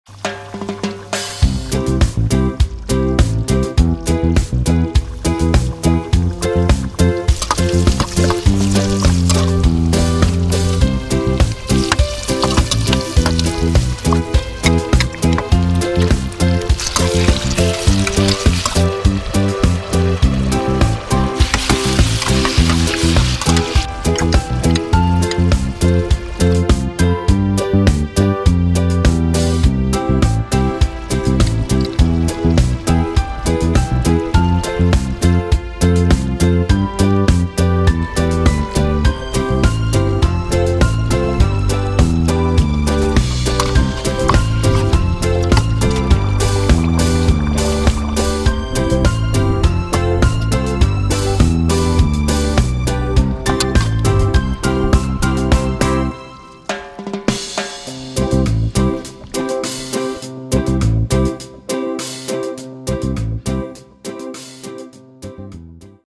🍷🍴 Ketjap Kip Masala
Een onweerstaanbaar duo: Ketjap en Masala. Ga in de keuken staan en maak er maar een trio van. Het recept Ketjap Kip Masala met boontjes is zo klaar. Wat je erbij serveert, mag je zelf weten: rijst, bami, nasi, spaghetti, Sandhia’s Roti of RotiChoti.